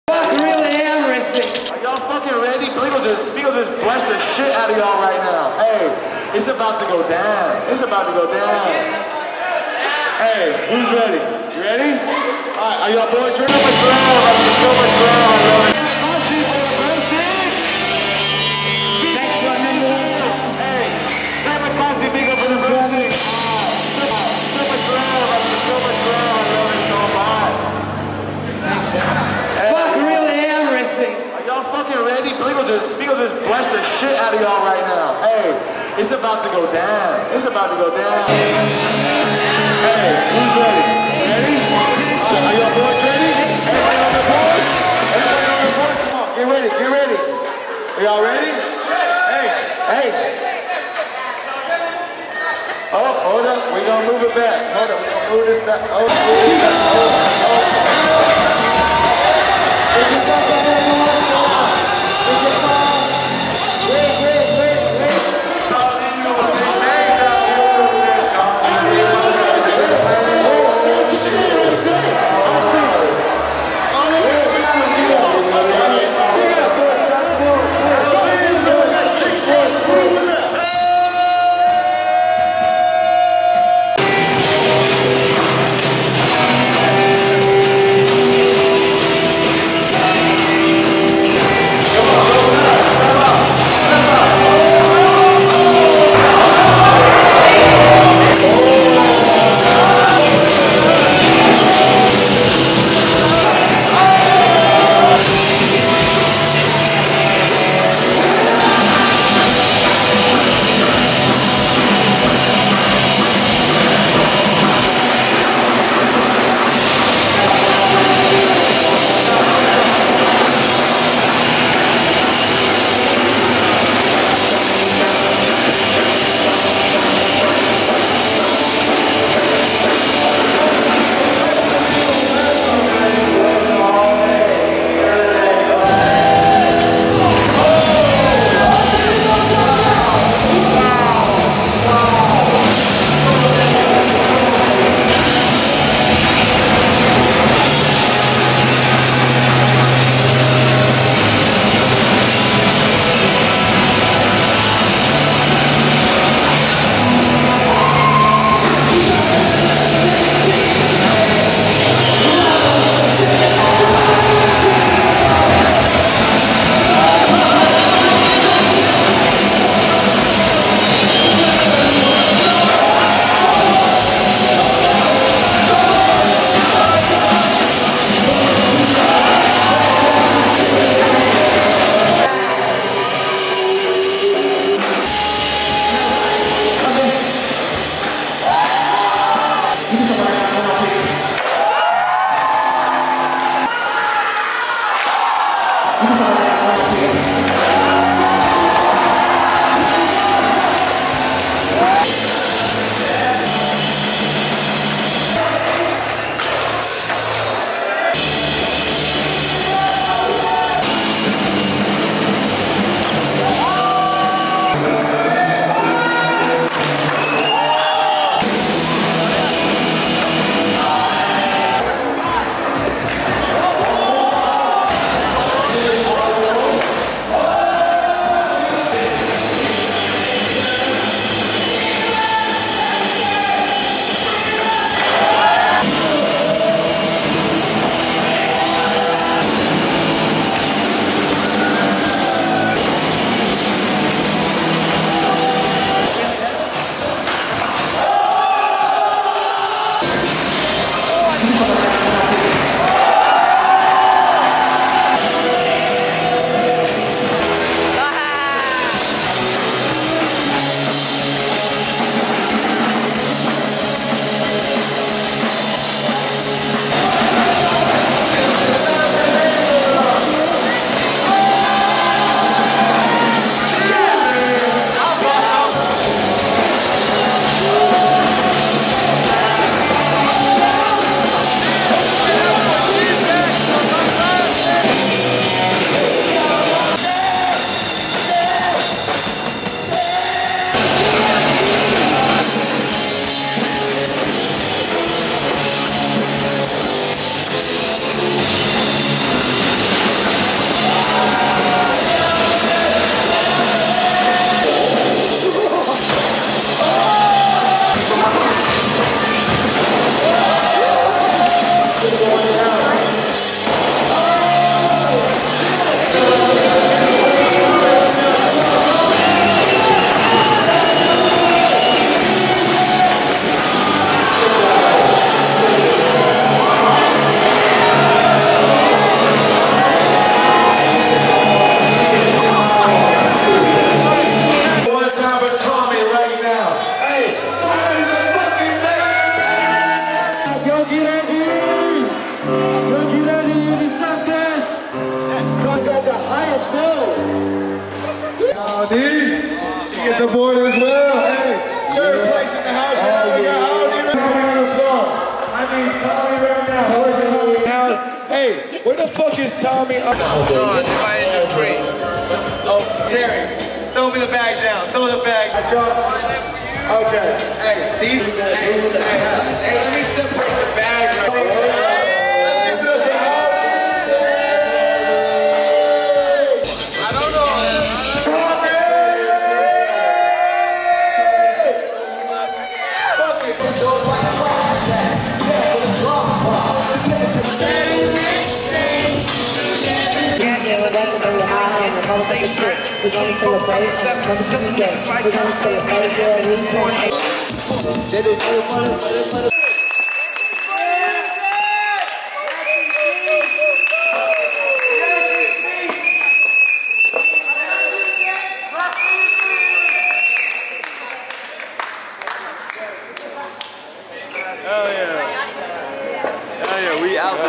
Tribute To Jake Phelps Video Bali 2019
The Ol' Man has brought so much joy and tears into the skateboarding community in the island of Gods. Here in Bali on the other side of the world doing it for the love of Jake. Tvstarkru

TRIBUTE BAND
LEARNING TO LIFE
PRISON AND GUNS
THE.FATHERS
THE MIDNITE
IRON HEAD
FUCK REALLY EVERYTHING
LORI MEYERS
DILATED EYE
WHITE SWAN
DJ BEAGLE1
DJ FUNKEN

Filmed By Victor Adianggara